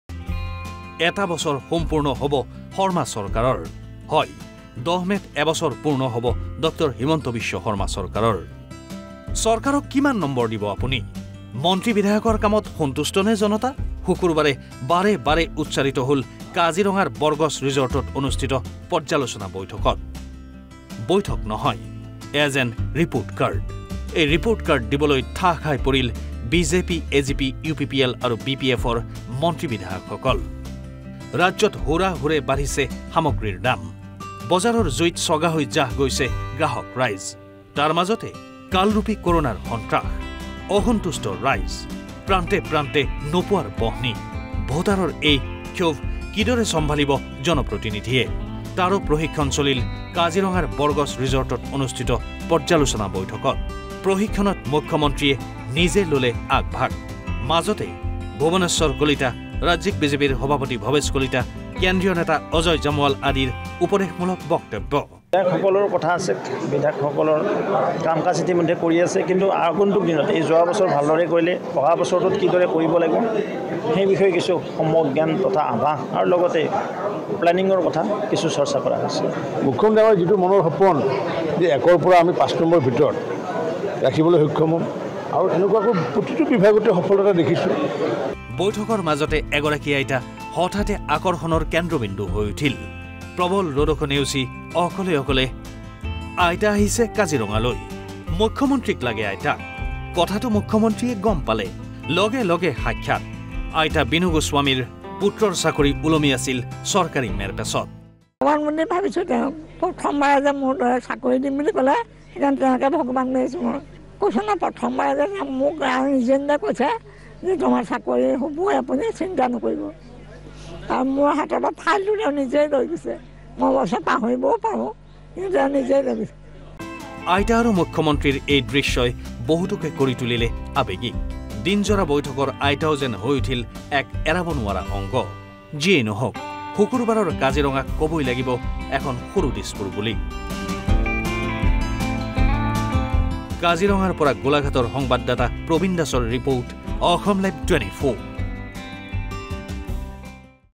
কাজিৰঙাত হঠাৎ চৰ্চালৈ আইতা বিনু গোস্বামী ৷ মুখ্যমন্ত্ৰীক কি ক’লে জানেনে?
কাজিৰঙাত হঠাৎ চৰ্চালৈ আইতা বিনু গোস্বামী
মুখ্যমন্ত্ৰীক কি ক’লে জানেনে?